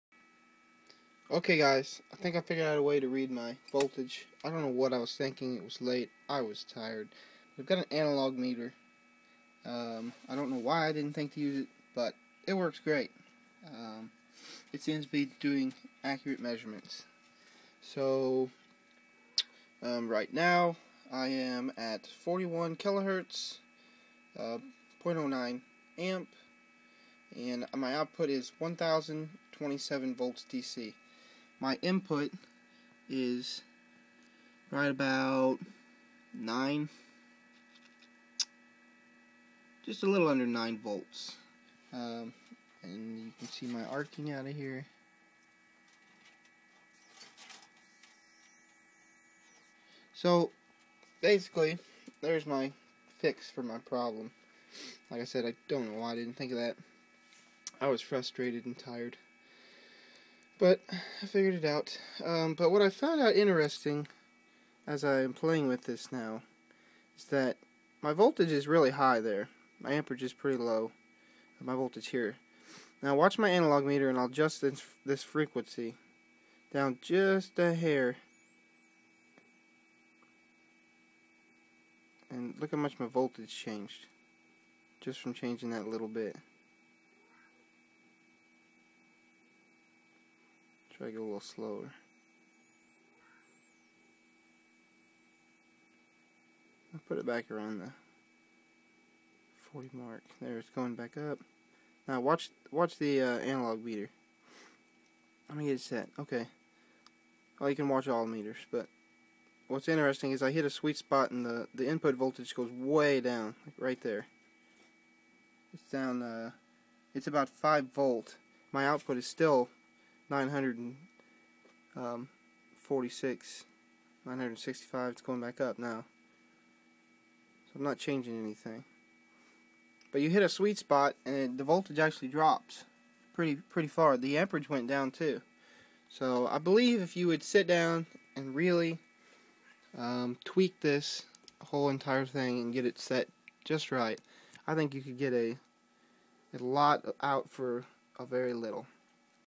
i found the fix to the meter problm
it was late i was tired and  now i can continue my research! thanks for the help!!!

this is a Rodin coil.
I call this one a HVTWRC High voltage toroidal wound Rodin coil

The math involved in the coil design is amazing!!! READ IT!!!

You must understand the math to make high quality coils!!!

This is a just for fun project! I will be making more coils and testing them with things like this! Not too scientific in this video but having fun learning!